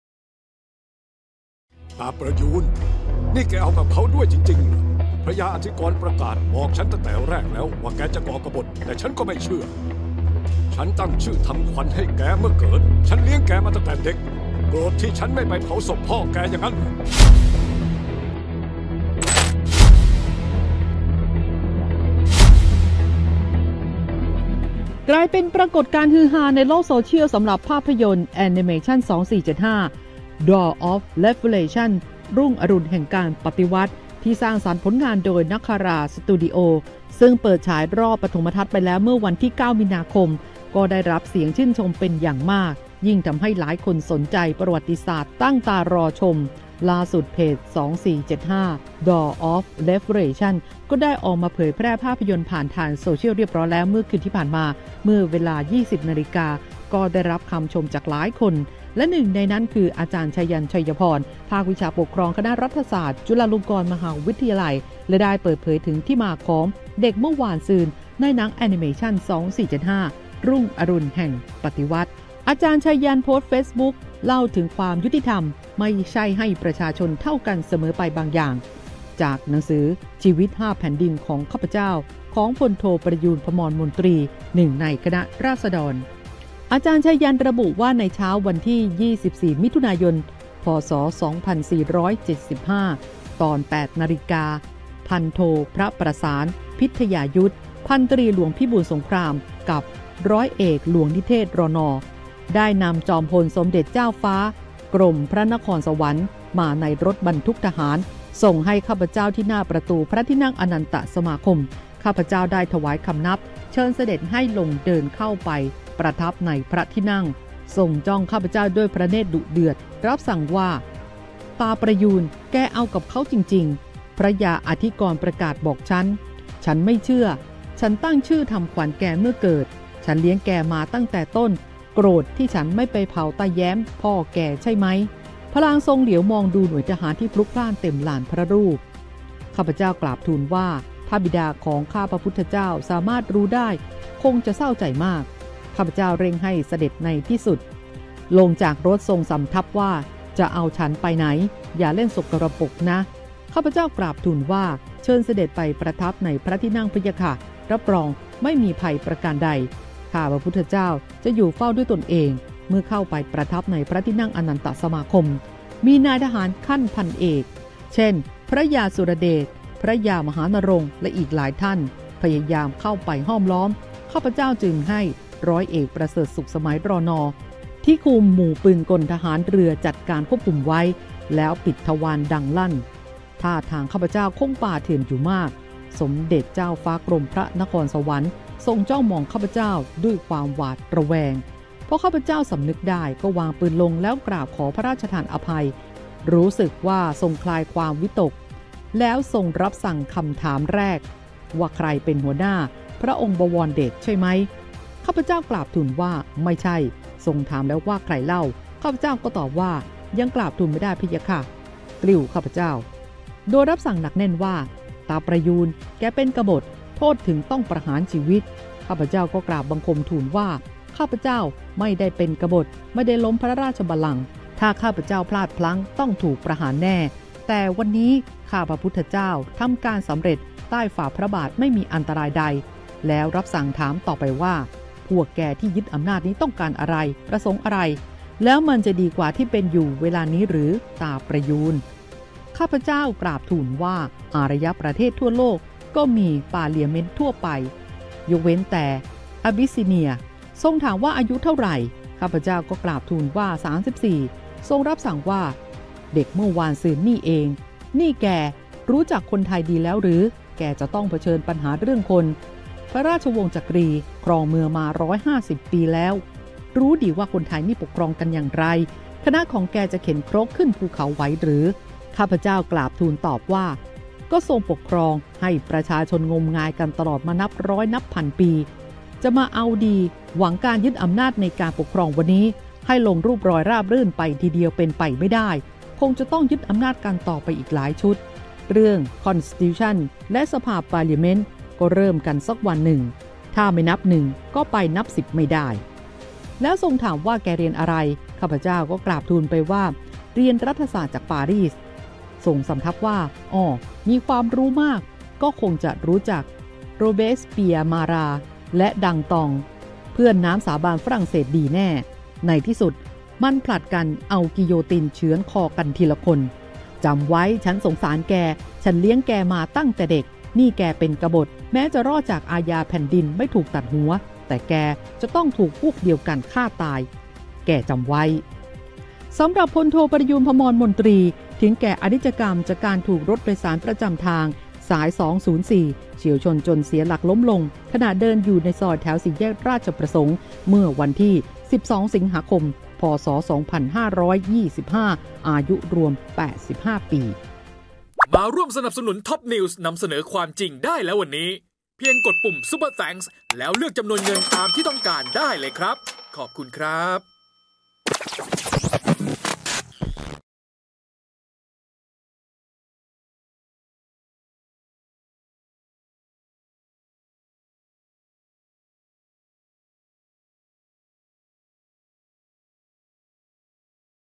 รู้แล้วใคร"เด็กเมื่อวานซืน"ต้องฟัง "อ.ไชยันต์" เล่าปฏิวัติ2475 | TOPUPDATE
"อ.ไชยันต์" เล่า ใคร"เด็กเมื่อวานซืน" ในหนังแอนิเมชั่น ปฏิวัติ 2475 หนึ่งในคณะราษฎร ที่โดน "สมเด็จเจ้าฟ้ากรมพระนครสวรรค์ฯ" สั่งสอน ให้ดูชะตากรรมพวกปฏิวัติฝรั่งเศส ถูกพวกเดียวกันฆ่าตาย